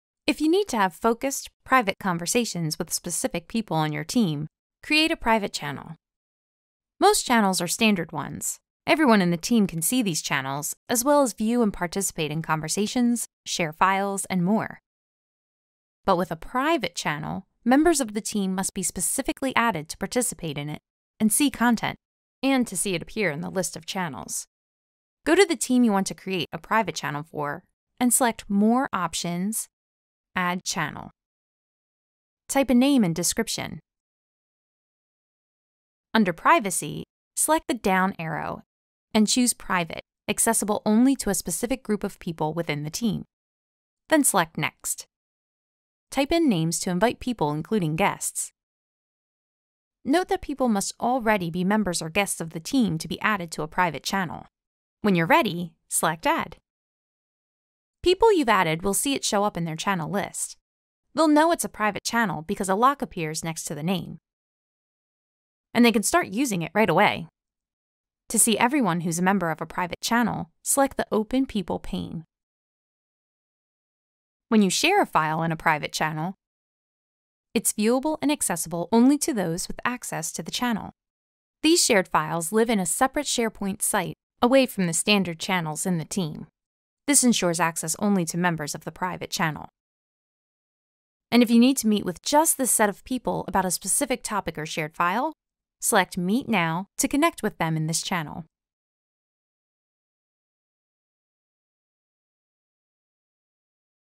How to create and use private channels in Microsoft Teams
With Microsoft Teams private channels, you can have focused, private conversations with people on your team. This Microsoft Teams tutorial will show you how to create private channels as you’re working remotely.

Most Teams channels are standard ones – meaning everyone on the team see them as well as view and participate in conversations, share files, and more. With a private channel, members of a team must be specifically added to it to participate, see content, and to see the channel appear in their list of channels.